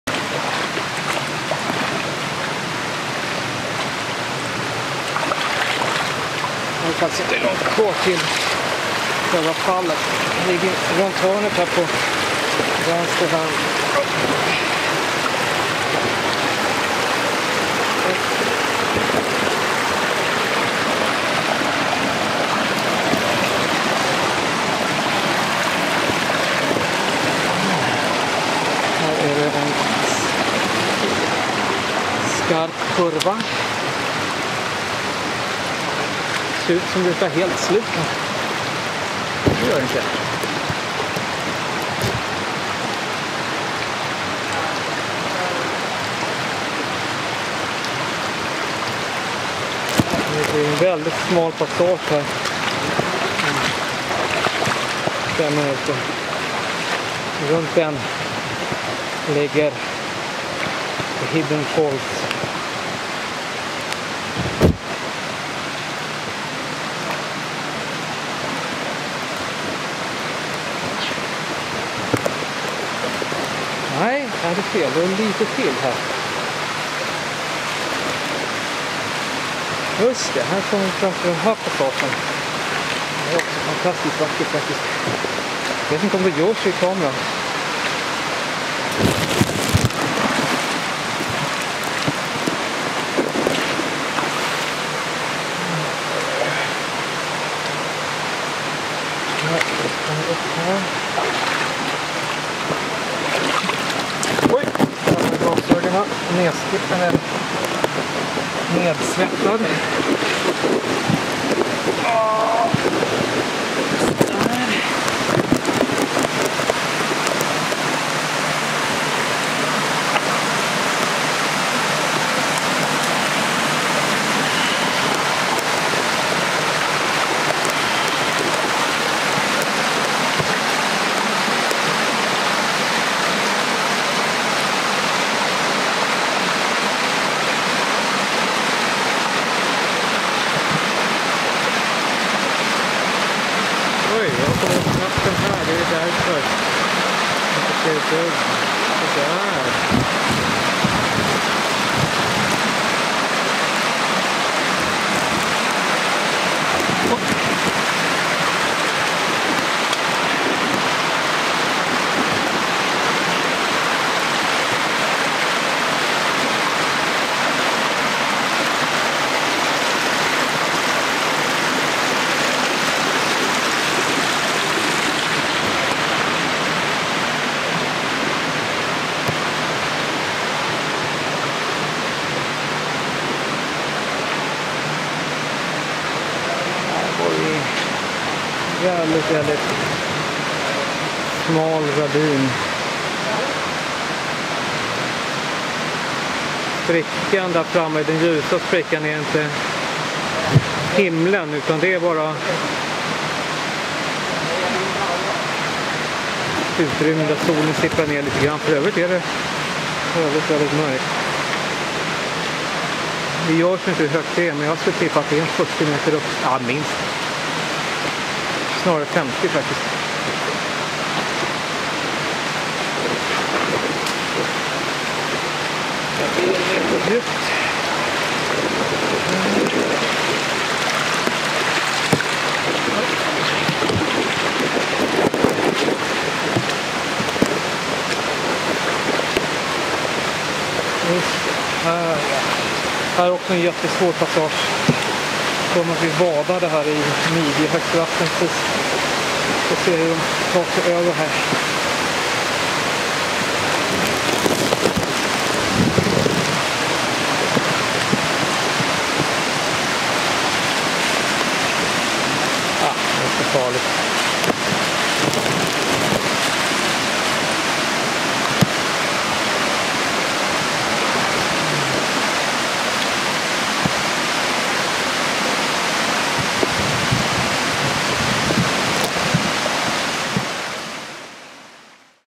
Philippines, Biliran province. Eco-trekking leading to the waterfalls in Balaquid.
The jungle trekking in the riverbed could be confusing and exhausting and the guys ahead look tiny when passing the narrow ravine. The walls are 40-50 meters (120-150 feet) high beside us and very impressive.